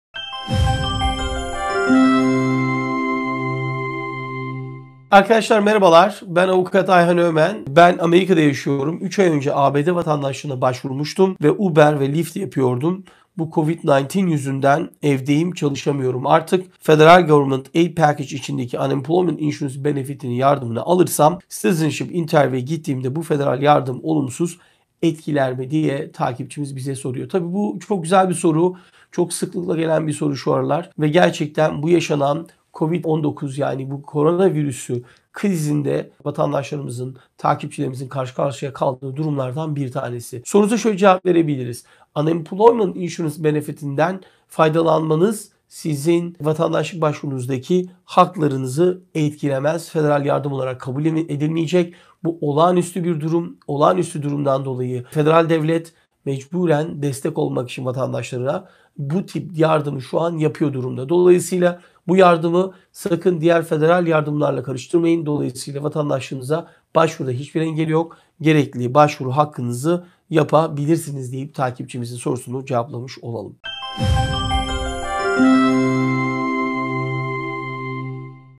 UBER VE LYFT YAPAN GREEN CARD SAHİBİ İŞSİZLİK YARDIMINA BAŞVURURSA VATANDAŞLIKTA SORUN OLUR MU ?
Soru: Ben Amerika’da yaşıyorum. 3 ay önce ABD vatandaşlığına başvurmuştum ve uber ve lyft yapıyordum. Bu covid-19 yüzünden evdeyim, çalışamıyorum artık ! Federal government aid package indeki unemployment insurance benefit yardımını alırsam, citizenship interview e gittiğimde, bu federal yardım olumsuz etkiler mi ?